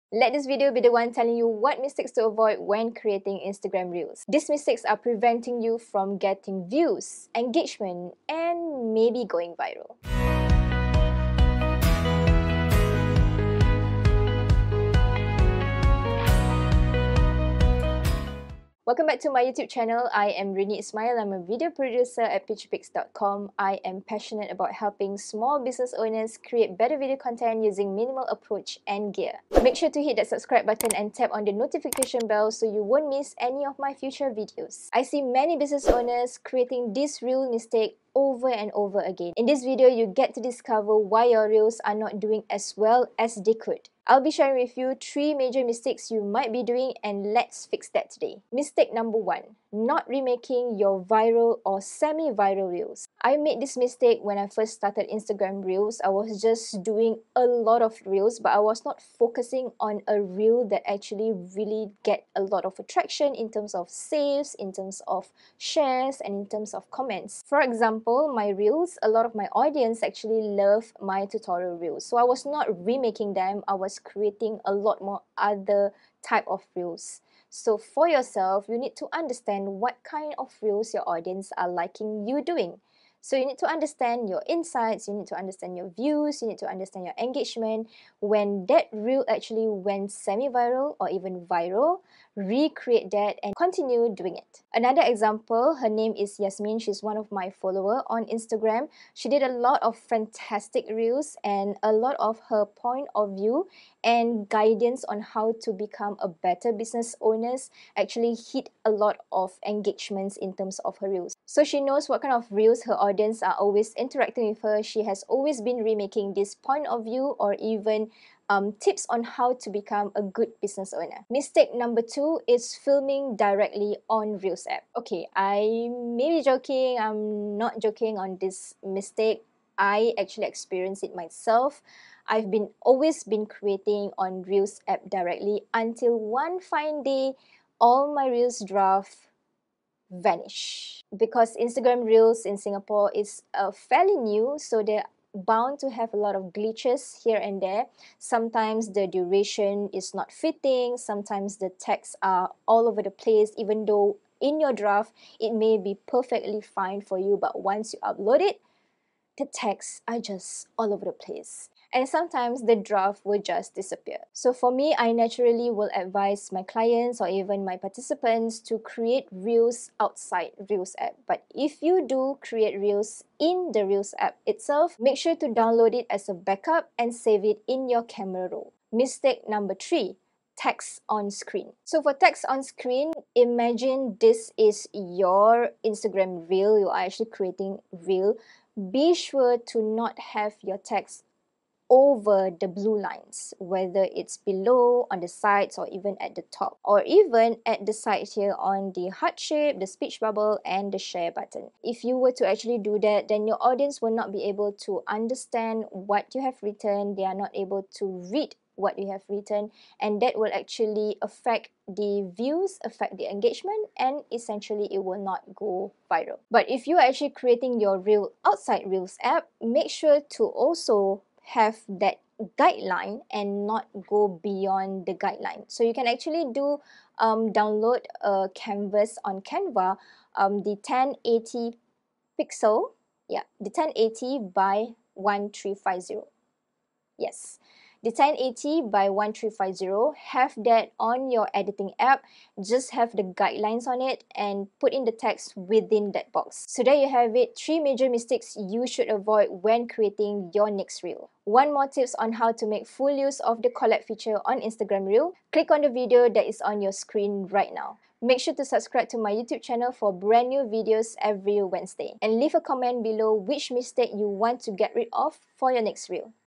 3 Reels Mistakes You Must Avoid
If you want to have more views on your Reels, watch this video!

Let this video be the one telling you what mistakes to avoid when making Instagram Reels.

There mistakes are preventing you from getting views, engagement, and maybe going viral.

🎥 Video equipment I use in this video :
Camera - Sony A7C
Lens - Sigma 16mm
Wireless microphone - Comica BoomX-D